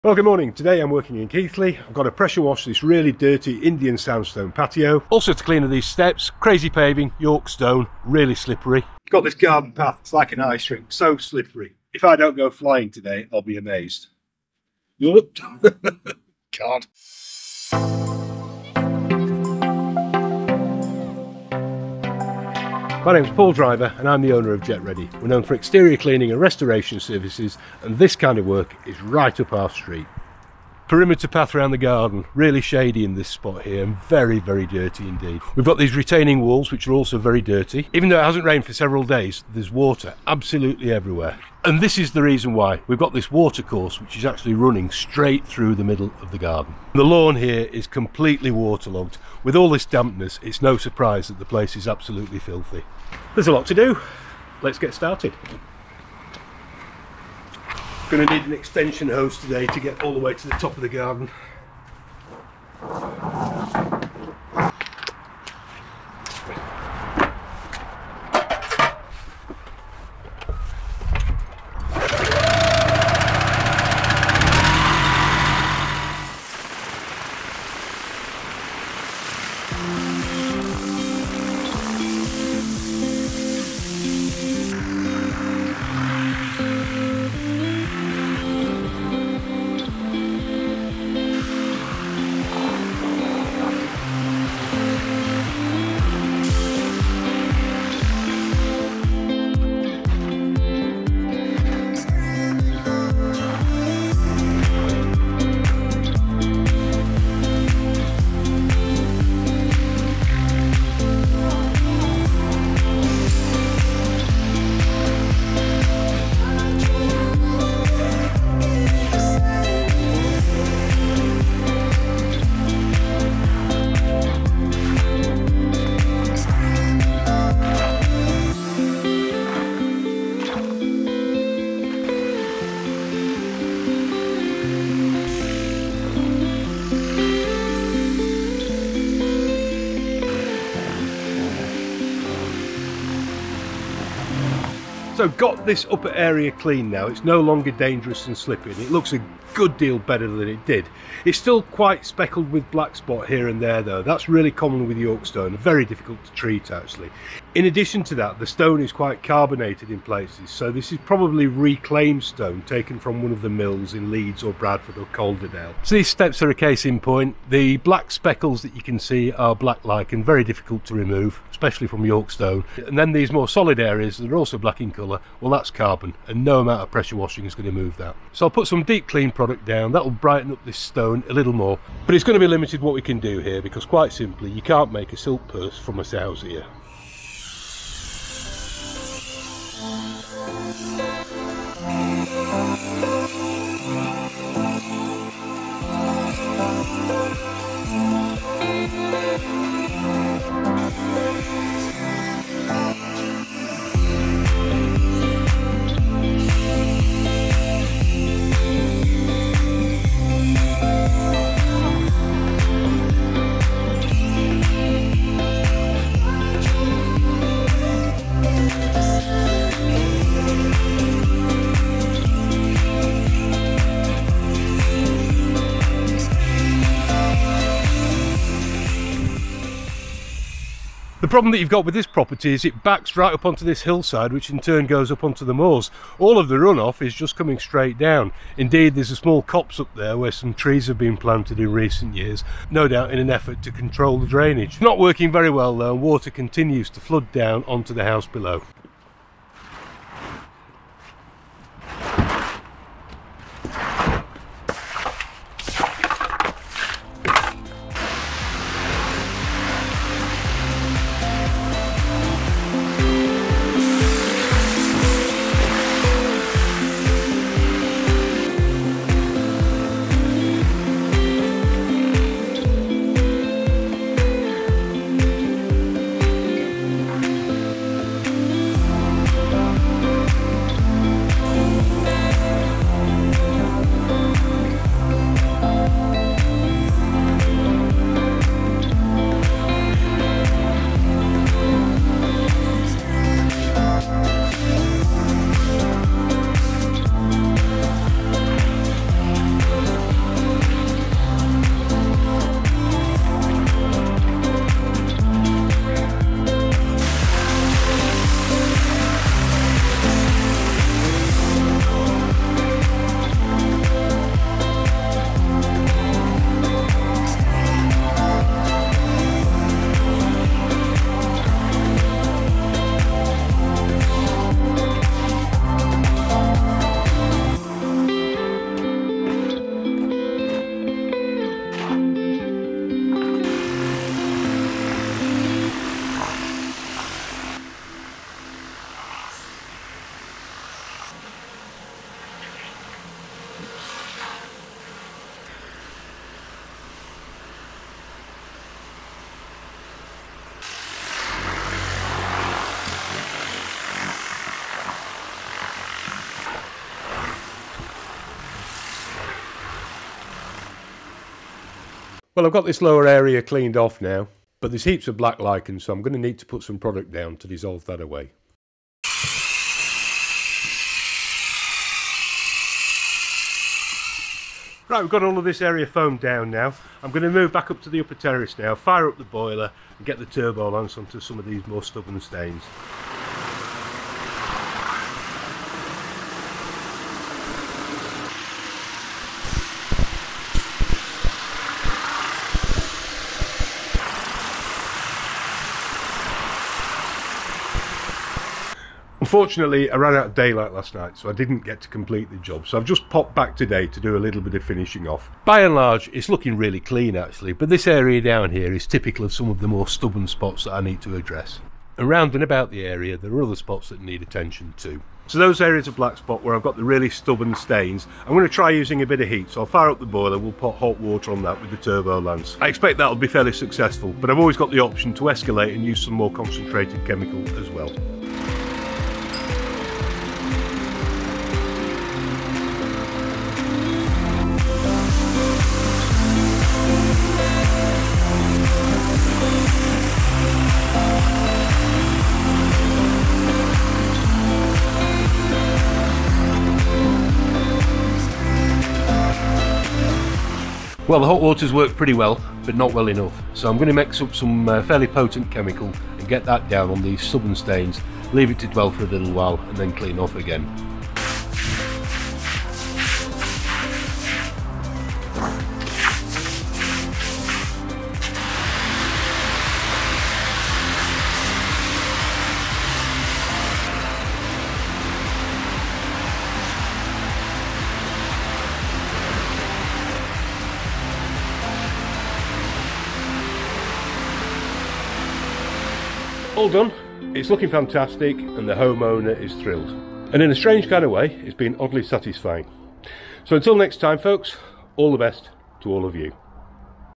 AMAZING PAVING MAKEOVER: But Why So Slimy? Pressure Washing, Keighley.
Paul nearly takes a tumble whilst pressure washing a slippery terraced garden.

To find out more about our services please get in touch.

JetReady - Pressure Washing and Exterior Cleaning Service
Serving Yorkshire and Lancashire